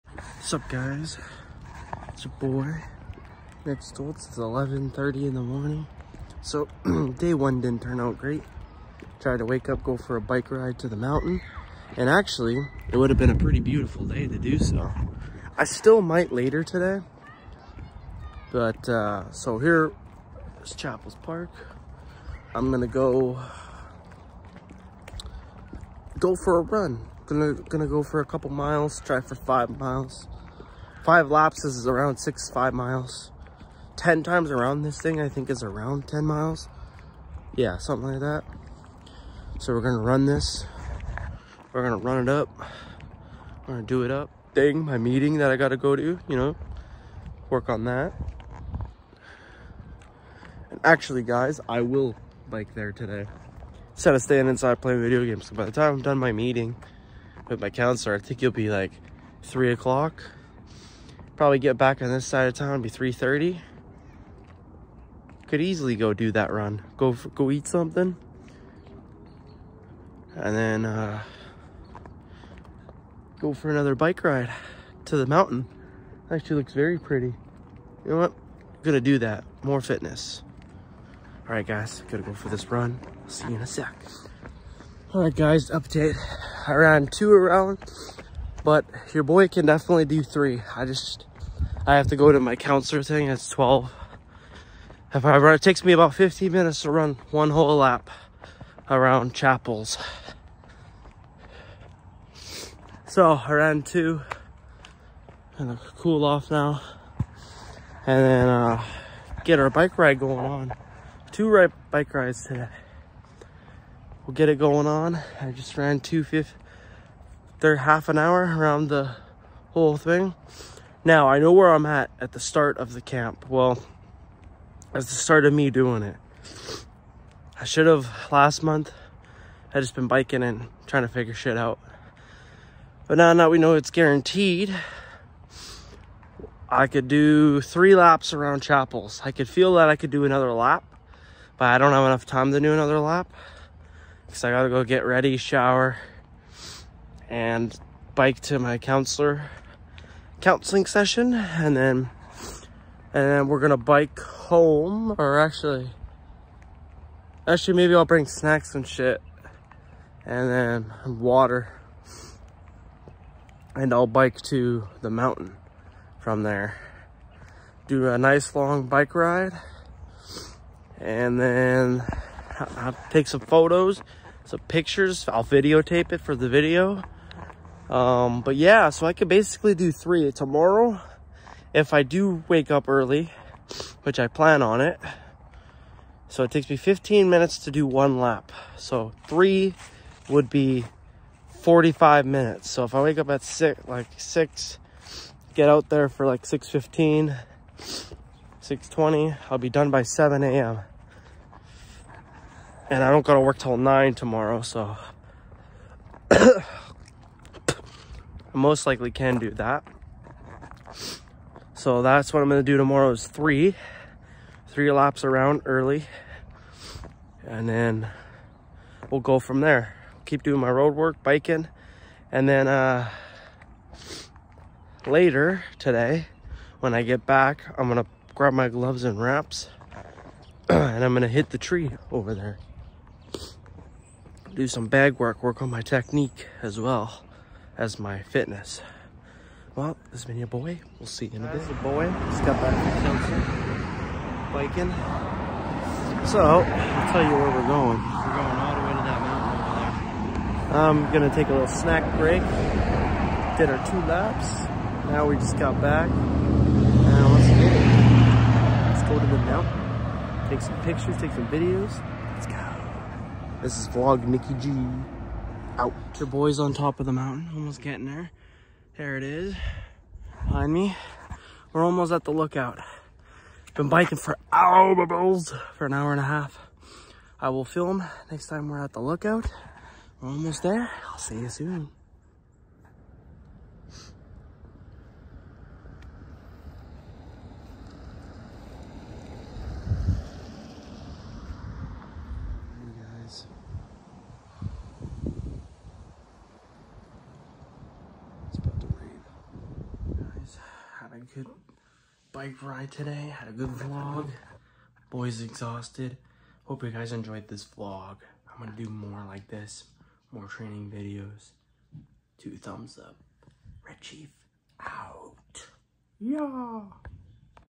Day, one of training, camp adventure to Mount McKay lookout!!
Starting a new blog series of my training, camp and adventures.